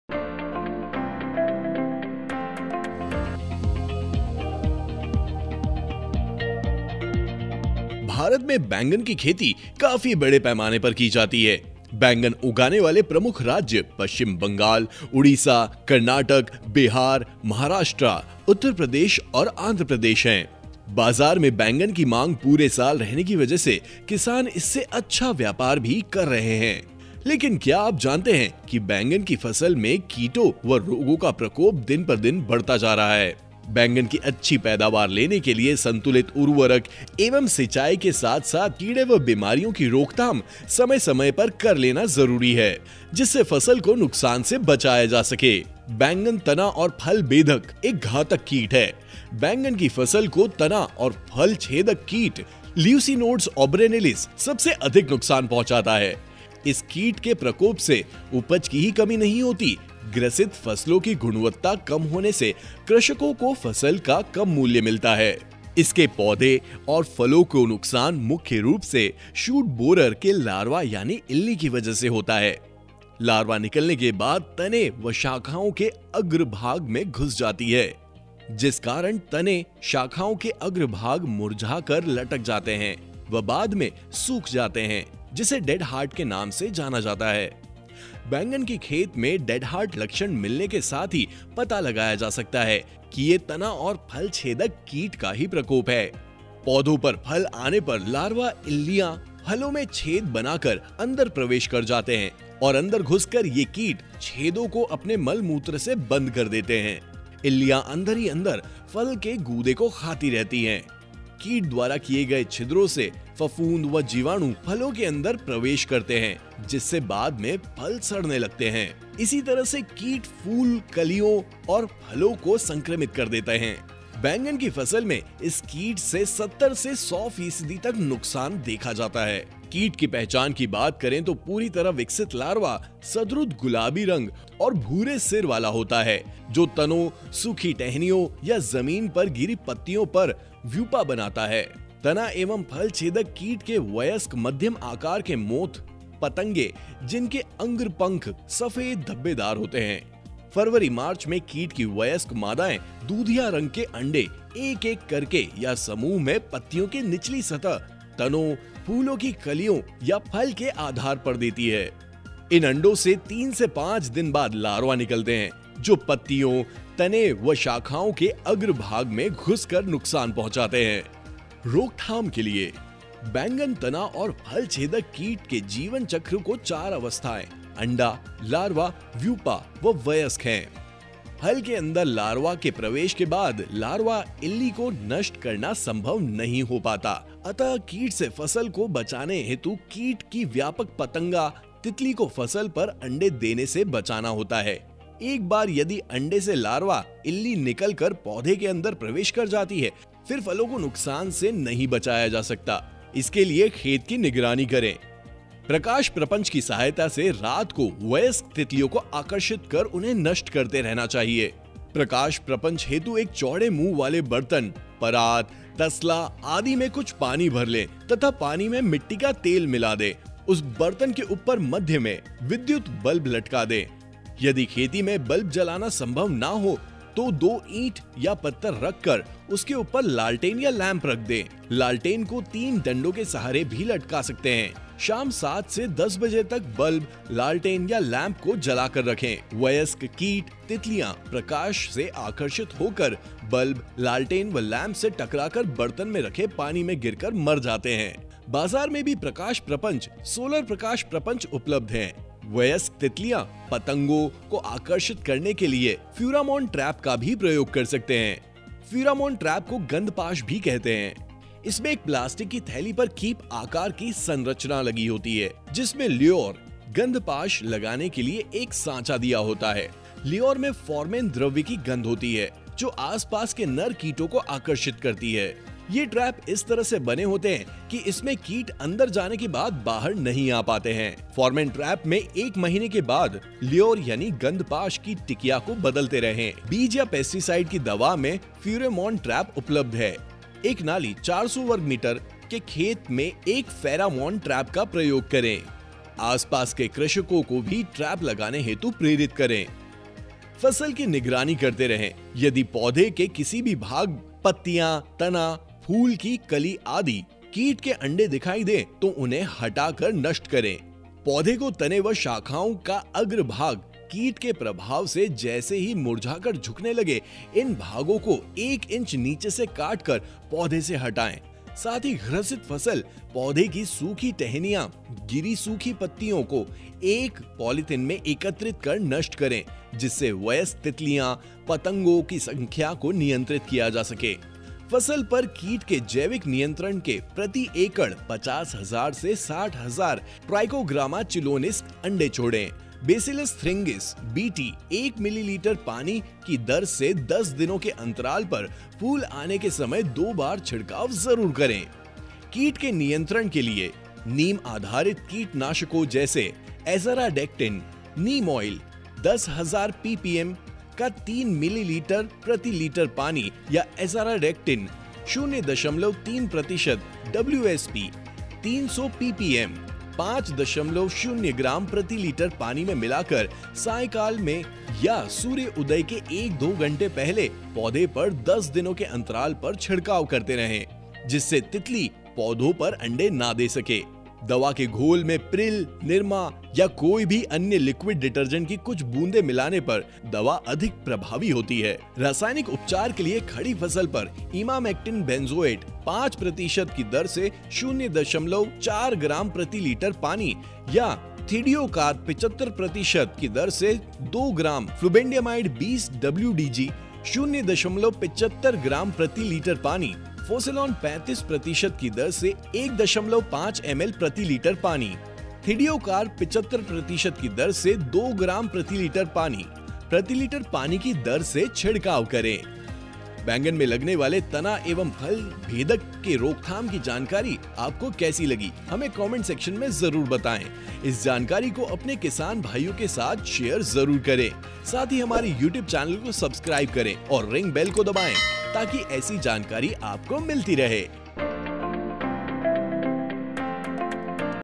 बैगन को लगने वाले फल भेदक से कैसे बचाएँ? जैविक और रासायनिक तरीके जानें।
उपज YouTube चैनल में आपका स्वागत है।

बाजार में बैंगन की मांग पूरे साल रहने की वजह से किसान इससे काफी अच्छा व्यापार भी कर रहे हैं, लेकिन क्या आप जानते हैं बैंगन की फसल में कीटों व रोगों का प्रकोप दिन पर दिन बढ़ता जा रहा है।

फल भेदक ऐसे ही कीटों में से एक है जो की बैगन की फसल को काफी नुक्सान पहुंचाता है।

आज जानेंगे की कैसे हम इस कीट से अपनी बैगन की फसल को बचा सकते हैं।